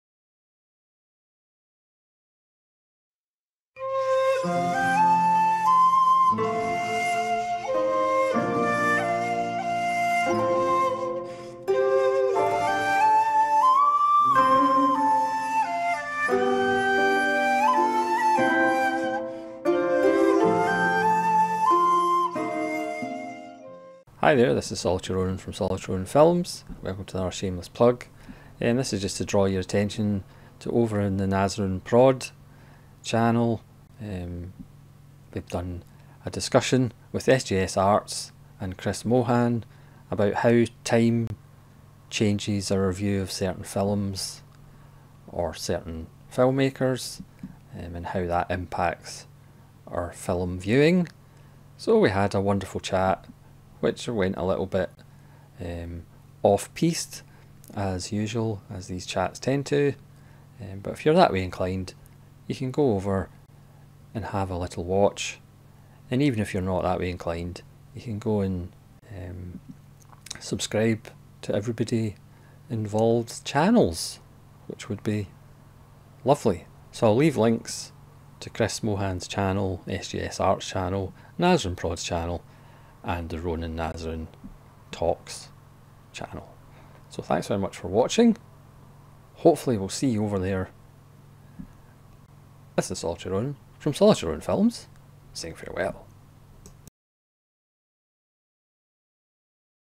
SHAMELESS PLUG:RONINAZARINTALK w/SJSARTS& CHRIS MOHAN
Over on Roninazarintalks channel we discuss how time and aging has affected our view of films and directors. Check it out here-